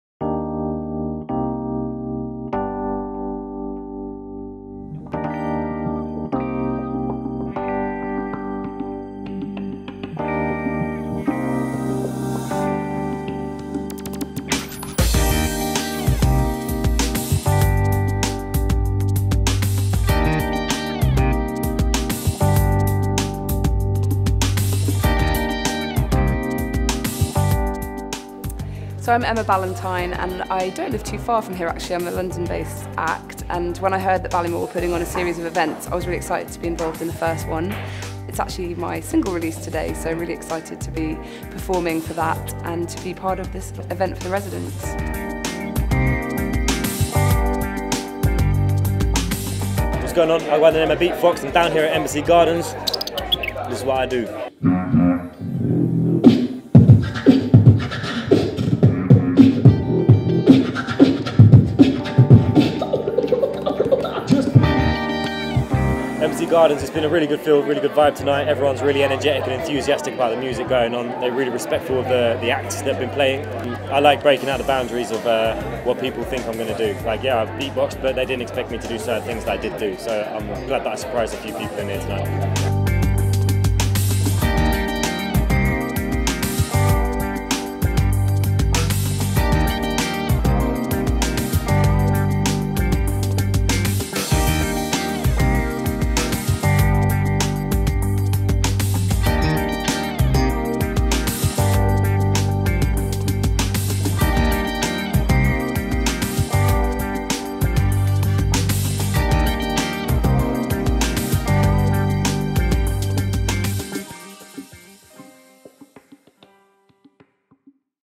EG Music Fridays
An intimate music gig at EG:LE with performances from folk artist, Emma Ballatine, jazz singer Iza Reni, singer songwriter, Nick Howe and hosted by comedian Marc Burrows.